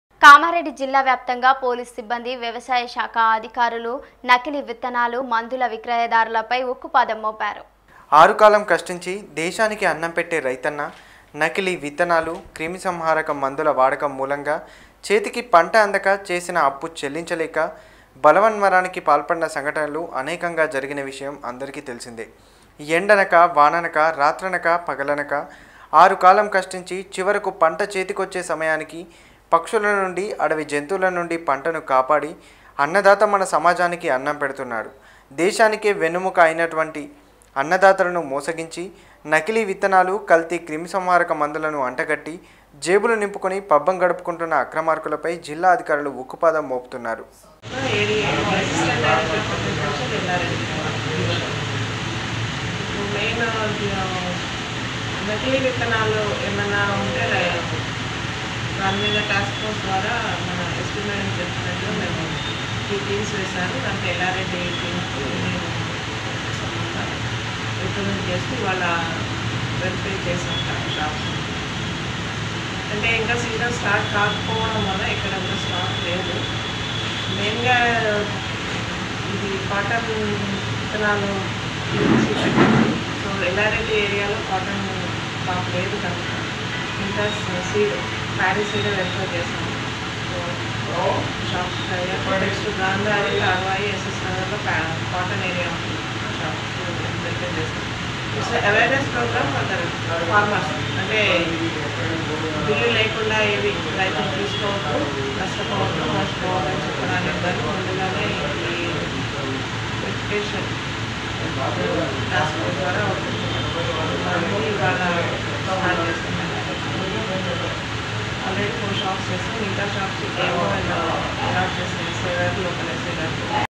TS24 News నకిలీ విత్తనాలు,కల్తీ క్రిమి సంహారక మందుల విక్రయాలపై ఉక్కుపాదం.
విత్తనాలు,కల్తీ క్రిమి సంహారక మందుల విక్రయాలపై ఉక్కుపాదం.